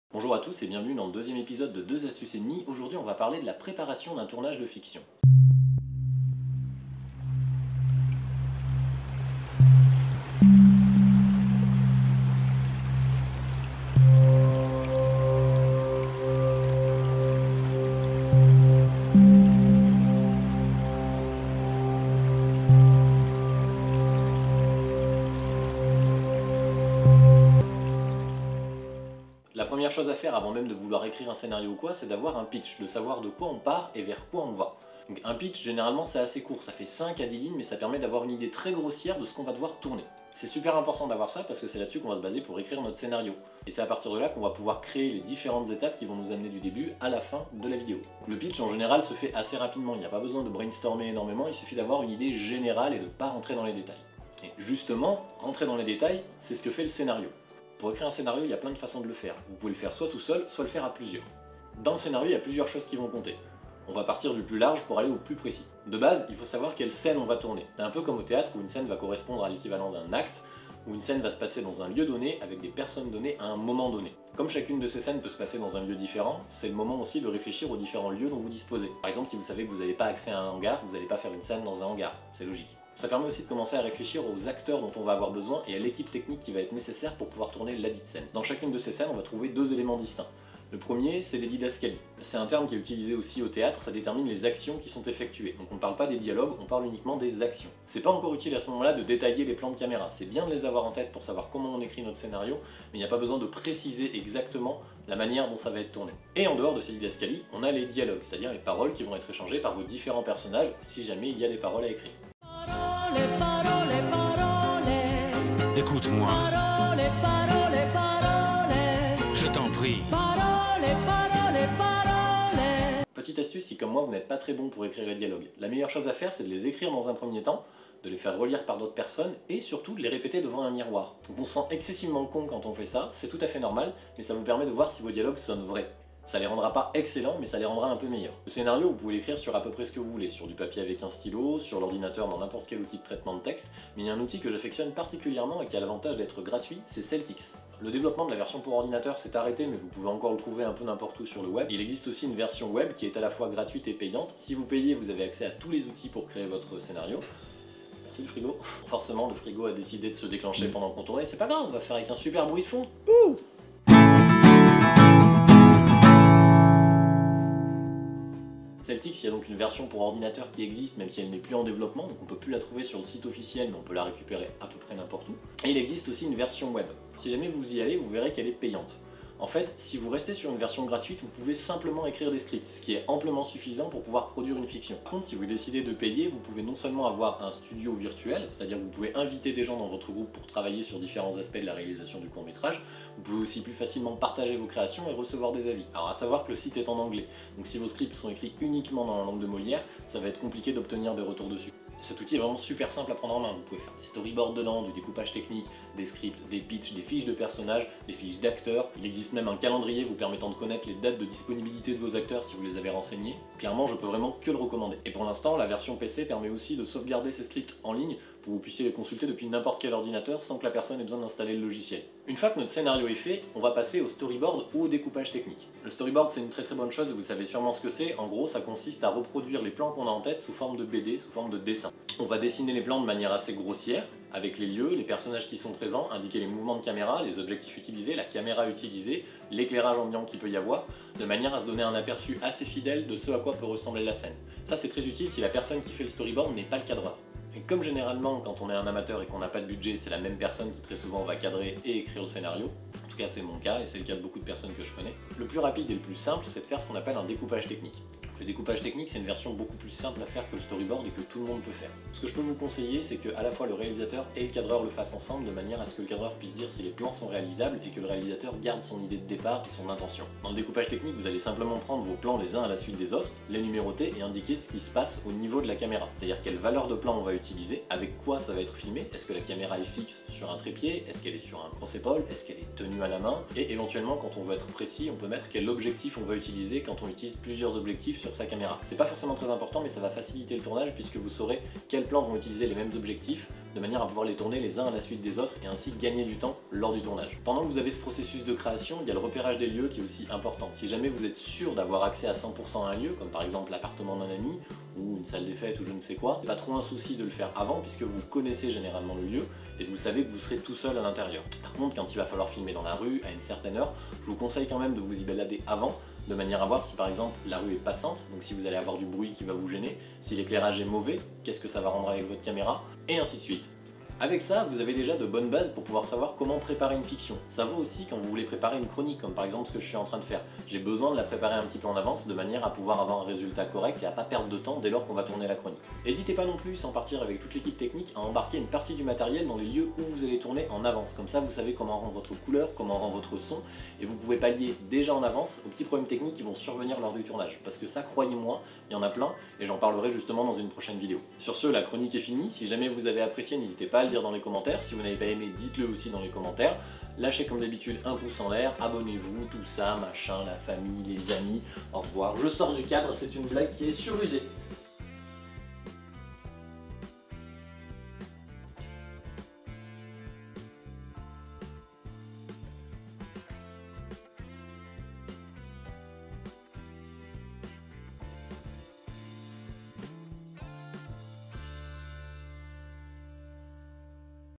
PRÉPARER LE TOURNAGE D'UNE FICTION - DEUX ASTUCES ET DEMI #2
Quand on veut tourner de la fiction, on ne sait jamais par où commencer. On croit souvent qu'avec une caméra, un micro et de la bonne volonté on peut y arriver mais il y a plusieurs étapes à ne surtout pas négliger avant d'attaquer le tournage ! Et comme je suis cool, je te file des astuces pour t'aider à te préparer.

De rien.

Retrouve moi sur les internets.